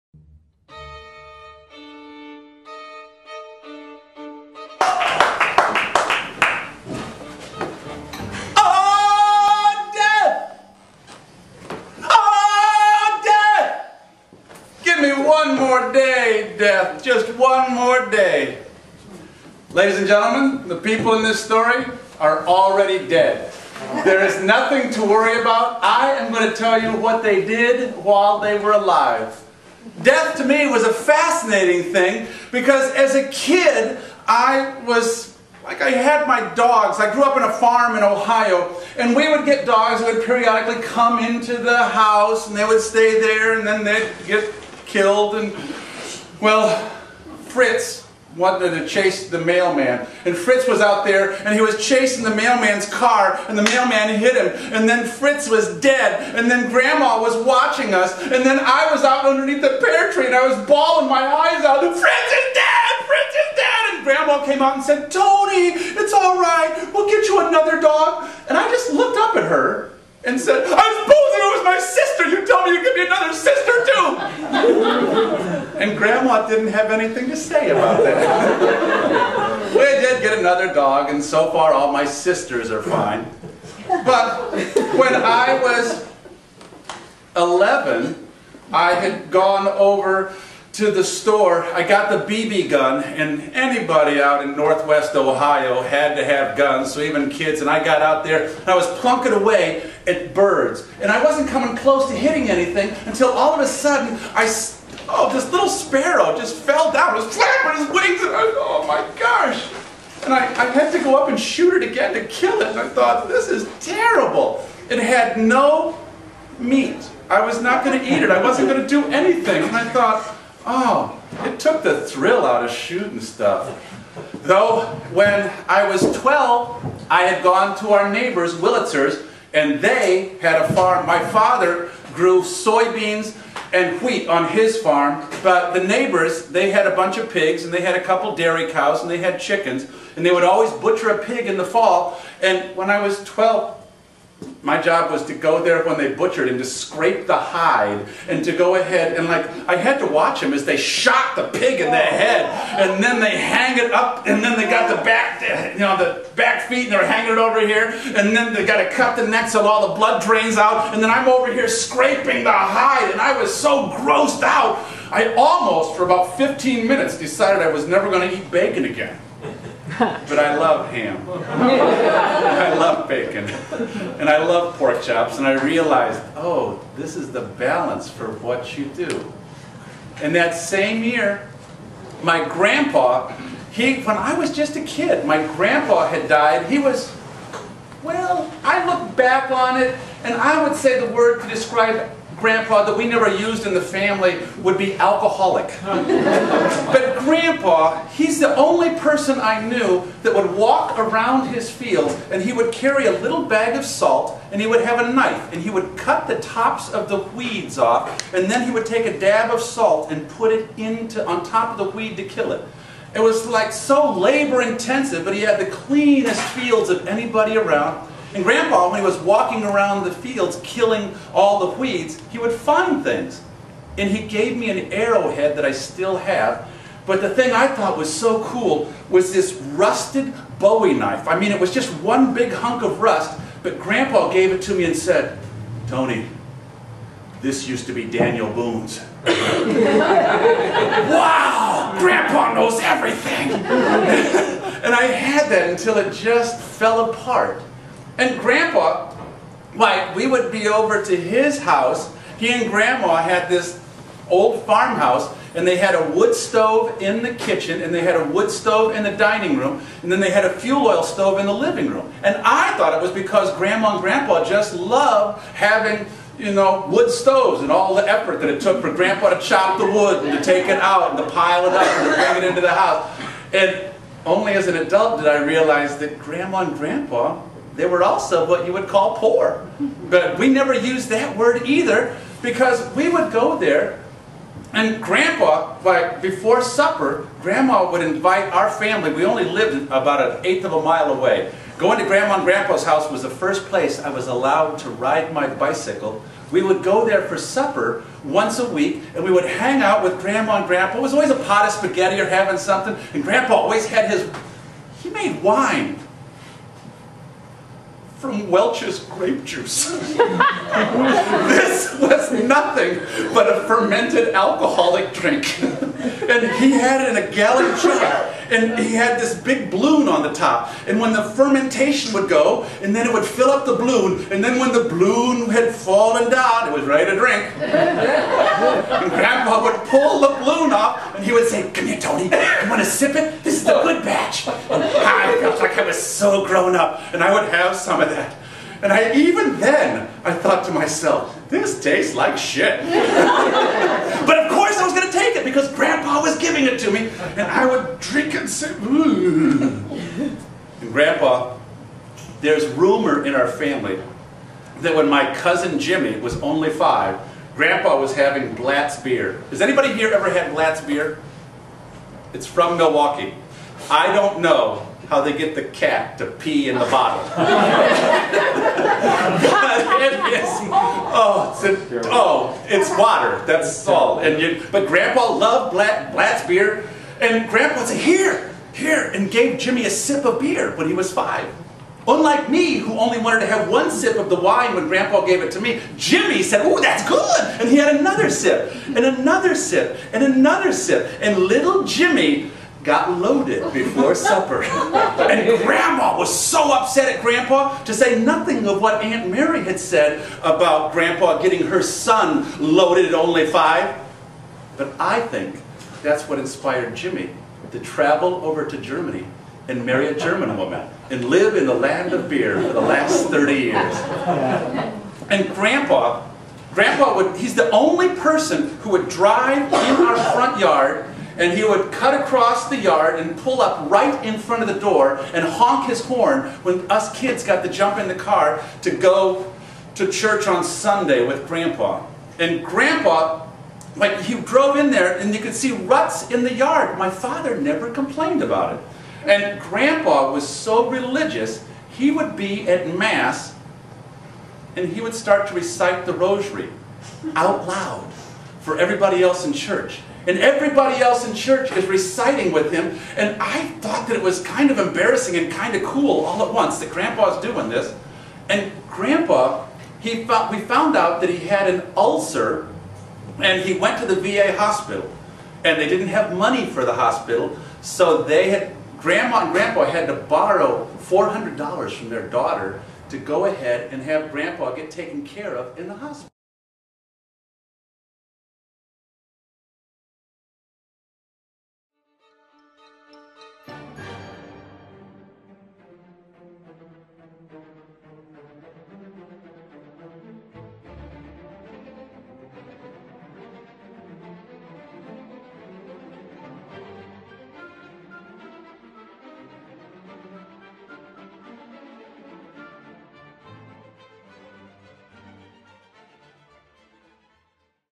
Tony Toledo@ Lily Pad June 11th 2010
An evening of personal narratives tall, short and otherwise some were stranger than fiction but all were true. by massmouth storytellers. organized by H.R.Britton of Overcoat Theater •  Tickets at the door - $10 dollars
June 11th,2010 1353 Cambridge Street in Inman Square Cambridge from 7PM -- 9:30PM
Tony Toledo  has been telling stories to pay the rent since 1990.

HR Britton's most recent monologue, From Madison to Madurai: 134 Days in Mother India is a humorous and sometimes sobering look a Britton's four-and-a-half month stay in India
Andrea Lovett telling "Grilled Underpants" and other personal favorites. Norah Dooley tells all in "Charter School" is English word for 'gulag"  Elizabeth  Appleby Elizabeth Appleby advises you fasten your seatbelt  as she tells  If you lived here, you'd be homeDEAD now.

Tony Toledo Tony tells tales to elementary students, senior citizens and preschoolers, though usually not all at the same time.

Elizabeth  Appleby Elizabeth Appleby is a performer, writer and educator. Her one woman show Fritz Perls Is My (tor) Mentor was produced at The Marsh in San Francisco. Early on, she labeled her life a living joke.
Bios
H.R.Britton is new to Boston.  His new monologue about his time in India,
        "Melting in Madras" opens at the Piano Factory Theatre this Tuesday.

Andrea Lovett  Founding member of massmouth,stoyteller , teacher, and has kissed a lot of frogs. Not complaining, just saying.

Norah Dooley, Founding member of massmouth is a storyteller, author and educator. She has swallowed many flies.